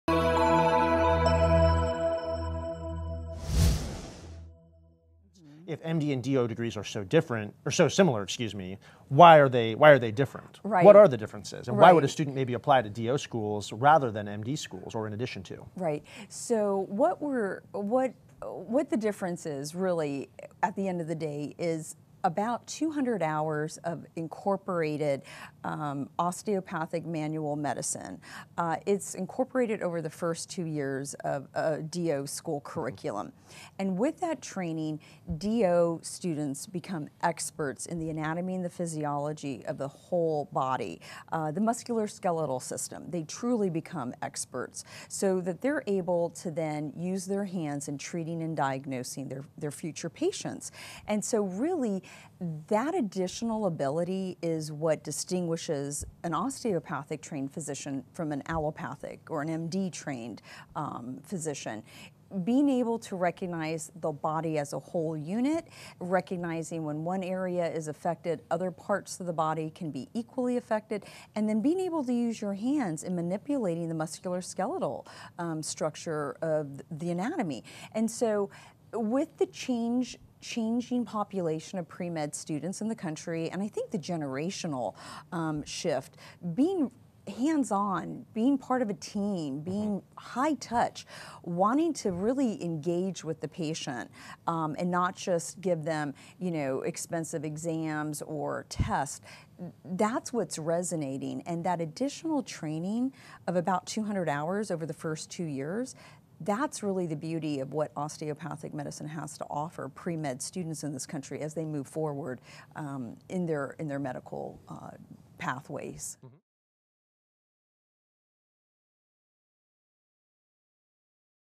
MD vs. DO: What is the Difference Between MD and DO? | Kaplan MCAT Prep
There are lots of options when it comes to medical school, but the first decision you need to make is what kind of degree to get: a DO (Osteopathic) or an MD (allopathic). Understanding the difference between the two philosophies will help you make an informed decision about where to go to medical school.

Pre-Med Life
Medical School Interviews
Medical Careers